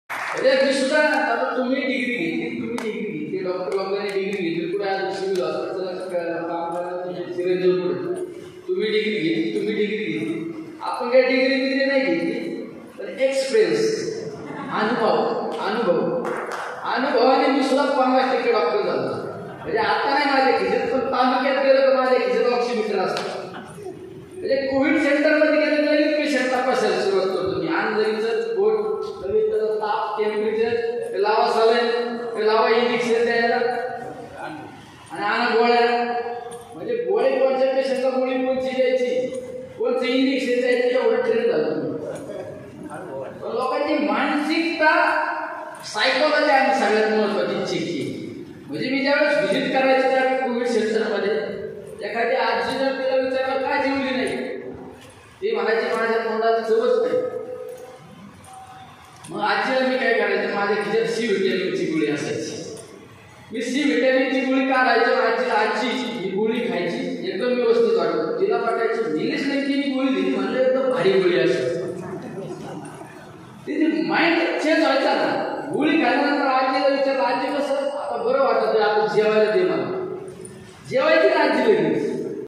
औषधांपेक्षा रुग्णांची मानसिकता बदलवून रुग्ण कोरोनामुक्त केले - आमदार निलेश लंके
“कोरोना काळात ८० टक्के मृत्यू हे केवळ भितीमुळे झाले. त्यामुळे या लोकांची भिती घालवण्याचं काम मी केलं. औषधांपेक्षा रुग्णांची मानसिकता बदलून जवळपास २५ हजार रुग्ण कोरोनामुक्त केले. त्यामुळे आता डिग्री न घेताही मी अनुभवातून 50% डॉक्टर झालोय.” असं वक्तव्य आमदार निलेश लंके यांनी केलं आहे. ते बीड जिल्ह्यातील शिरूर कासार येथे आयोजित कोरोना योध्द्यांच्या सन्मान सोहळ्यात बोलत होते.